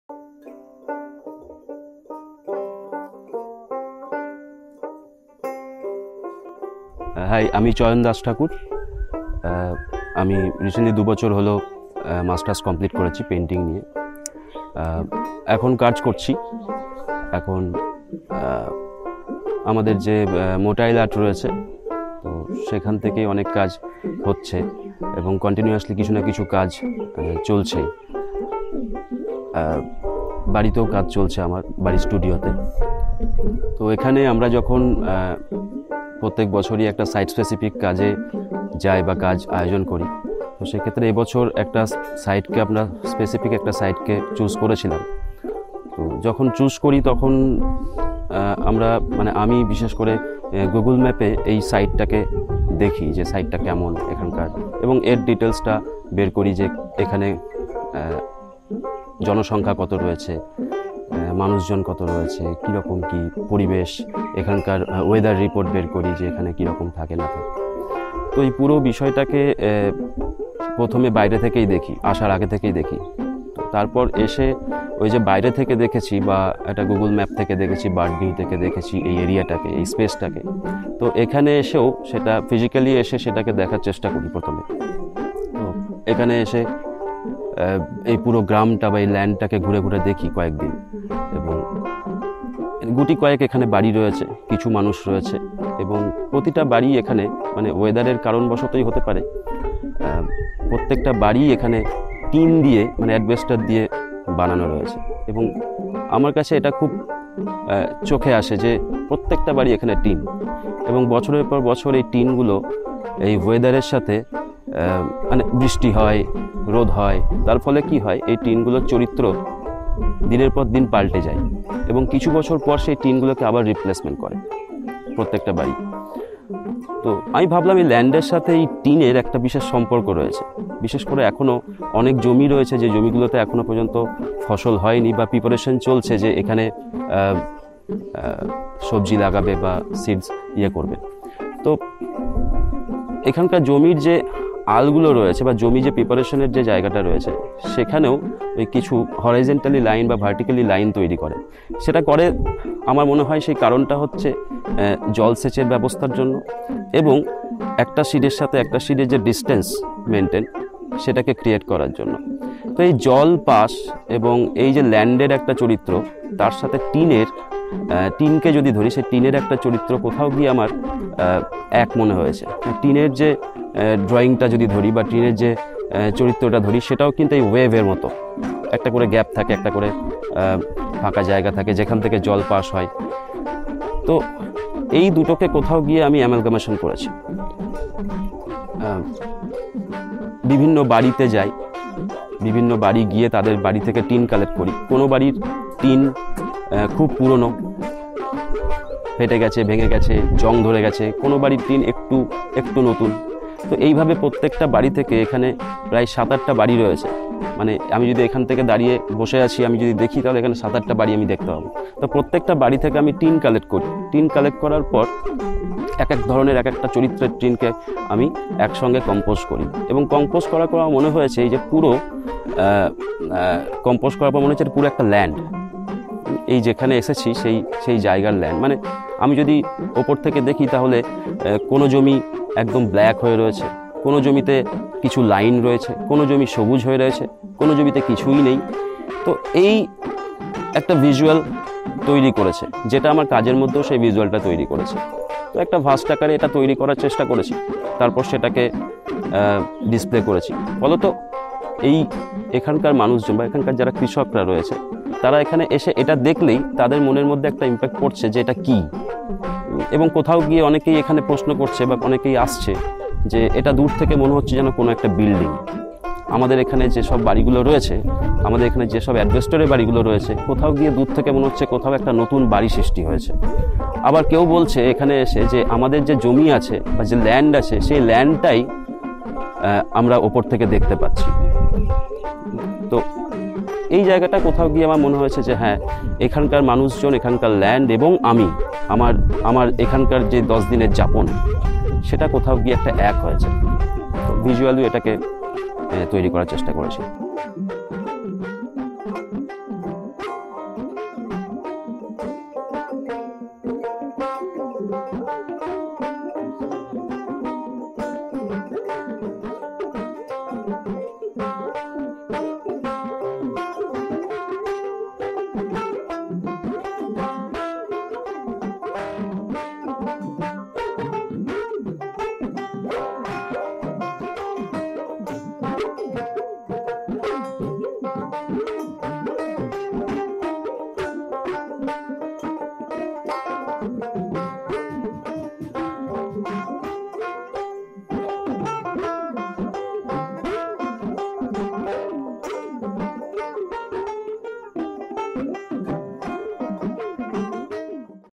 A Site-Specific Artwork by Artist Chayan Das Thakur || Rangamati Residency 2022
Chayan Das Thakur is an artist based in Suri, West Bengal. During the residency, he created a landscape of this village Rangamati by using some collected materials like waste asbestos from different people’s houses. Some are rusty some are not, he has assembled all asbestos as an emotion of the farmers. By attaching asbestos with each other it become a massive landscape which was reflecting my experience. He realized that landscape is divided into many small properties some properties are prepared for cultivation, some are already cultivated, and the soil has various colors like brown, black ochre yellow, and grey like asbestos has many colors. Later he has attached his presence by drawing with black color on it as an amalgamation of thought, self, land, and asbestos. That image has the potency to lead someone towards dimensions of thoughts that artist like to address as "Temporary Emotions".

A SITE-SPECIFIC ART RESIDENCY AT RANGAMATI 2022
Modernist art objects were transportable, nomadic, could only exist in the museum space, and were the objects of the market and commodification. Since 1960 the artists were trying to find a way out of this situation and thus drew attention to the site and the context around this site. The work of art was created on the site and could only exist and in such circumstances, it can not be moved or changed. The site is a current location, which comprises a unique combination of physical elements: depth, length, weight, height, shape, walls, and temperature. With this thought, Motile has gathered with some aspiring artists in Rangamati, West Bengal. The people here are involved in various agricultural activities as their foremost occupation. In this residency, we invite artists from different parts of India to explore, experience, and get connected with nature.  This initiative primarily celebrates an artist's connection with nature using materials found on the site. The concept is best understood in relation to historic earth or Land art and the evolving field of ecological art. The field is interdisciplinary in the fact that environmental artists embrace ideas from science and philosophy. The practice encompasses traditional media, new media, and critical social forms of production. Participants has interact and interchanged their thoughts with each other to get a better understanding of that period.